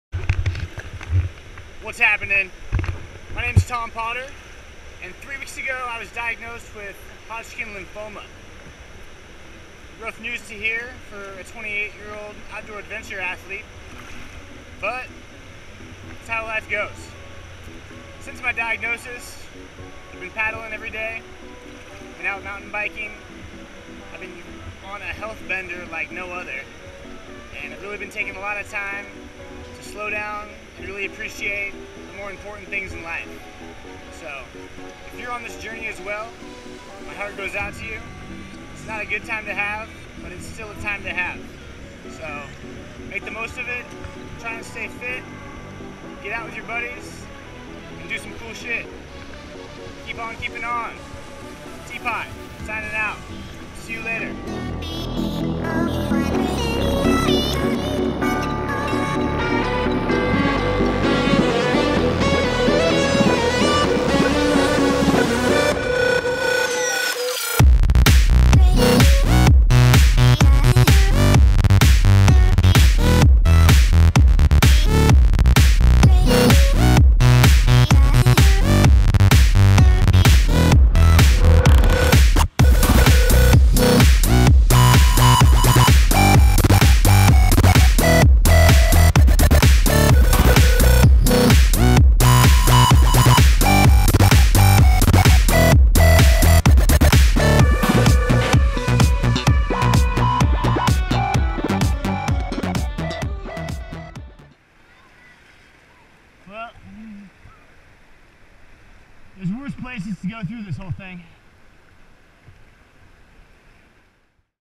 Whitewater Kayaking: Cancer Treatment
Obviously, whitewater kayaking isn't going to treat cancer. However, it will give you the freedom of exploration, play, and and mental break from anything else that may be going on in your life.